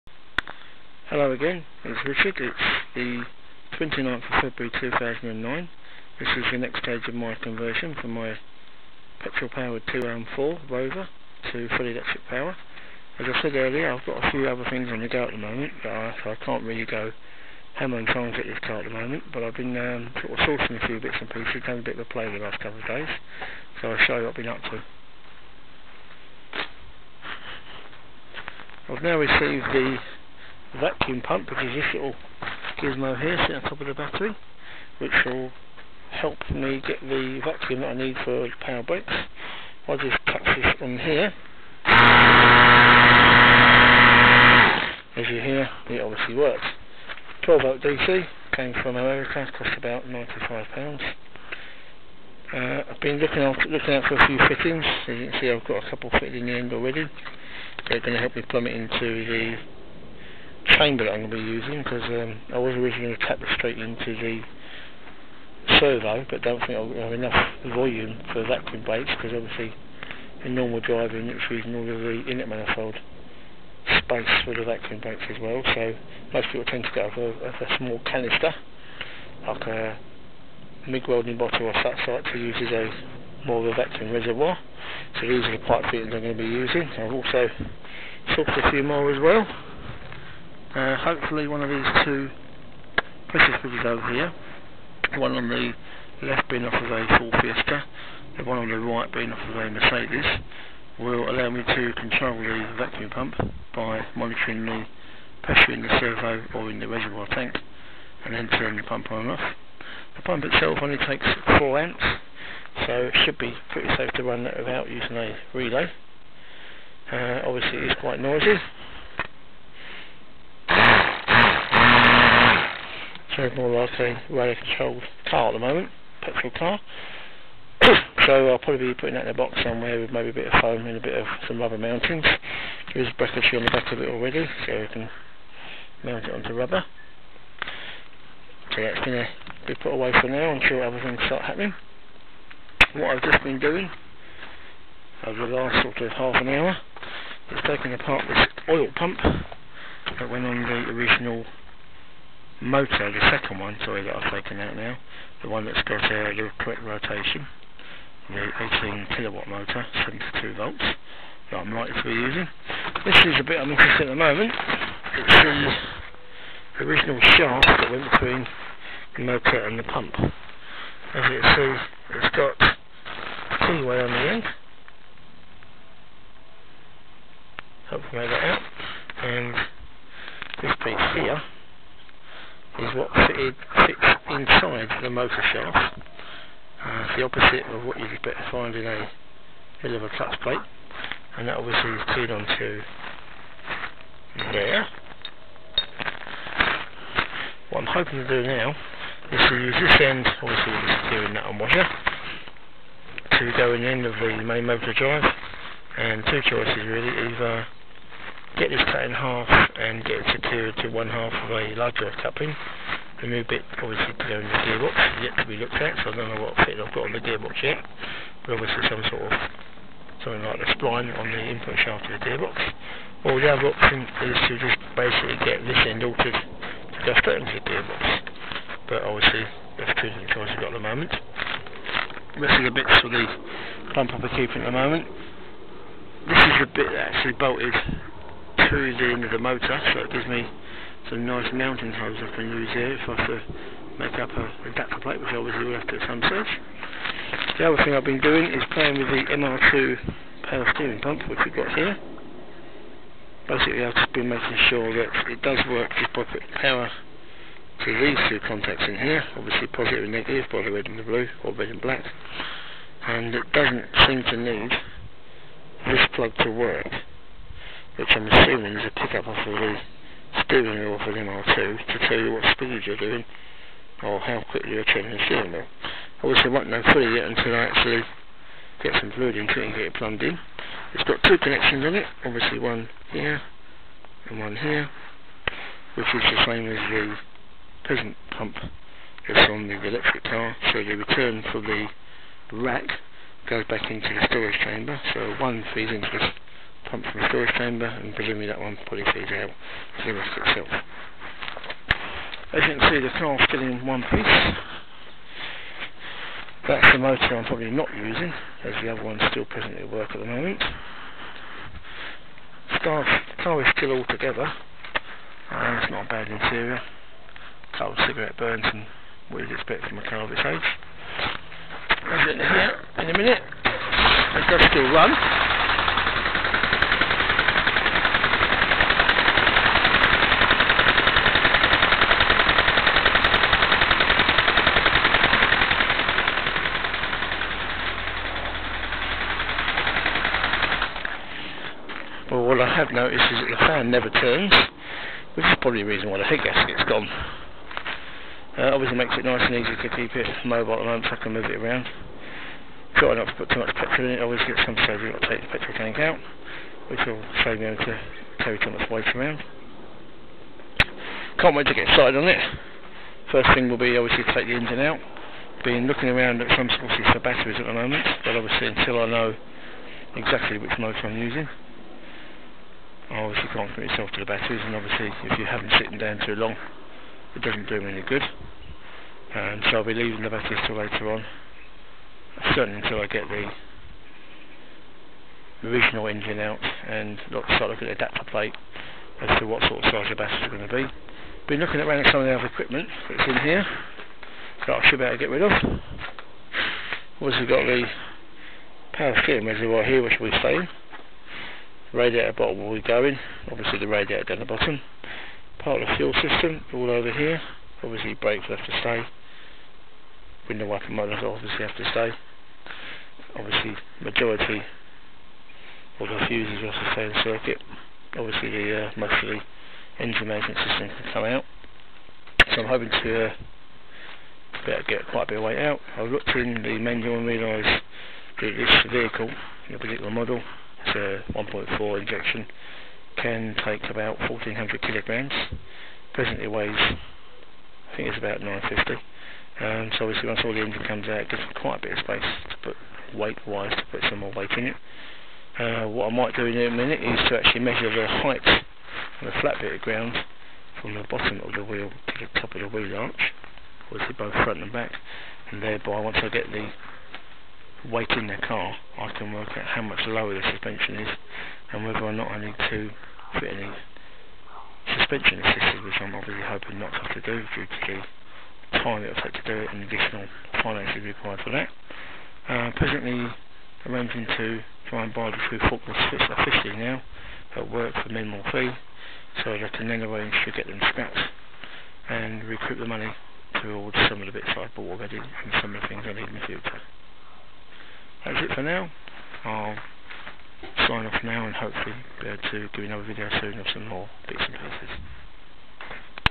Electric Car Conversion Rover Part 4
Starting to get pieces together. Motor coupling, Vacuum Pump and Electric Steering Pump